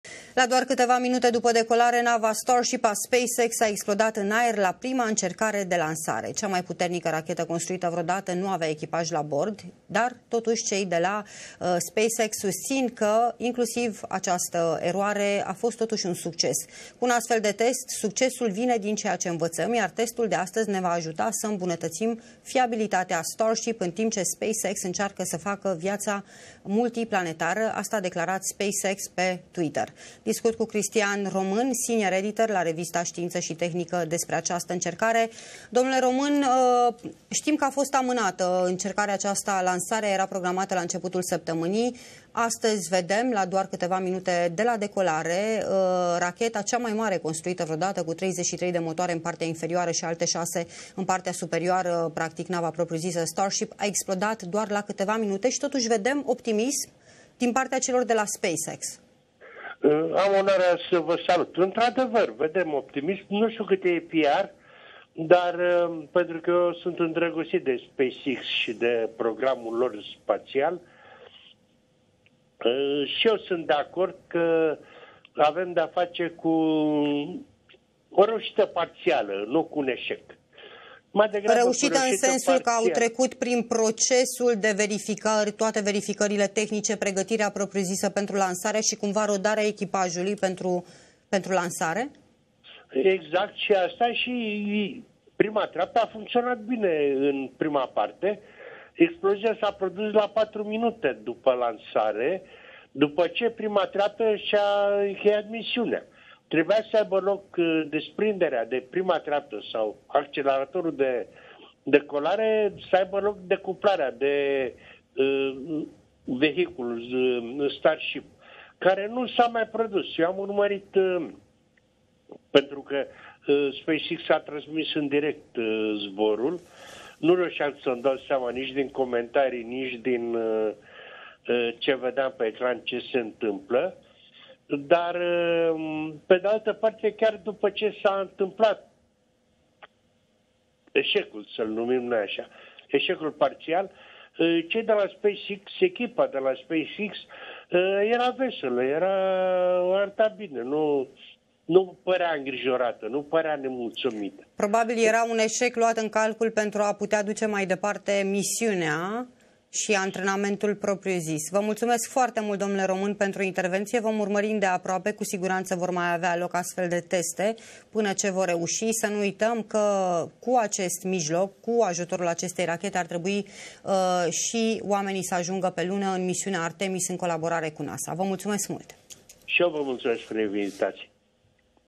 ATENȚIE: Racheta Starship de la SpaceX a explodat la scurt timp după lansare
➥ Racheta s-a desprins de sol, însă s-a dezasamblat în timpul zborului
➥ Cea mai puternică rachetă construită vreodată nu avea echipaj la bord
➥ Nava spațială a fost gândită de Elon Musk ca mijloc de colonizare a planetei Marte
➥ Cristian Român, senior editor Revista Știință și Tehnică, a dezbătut la Aleph News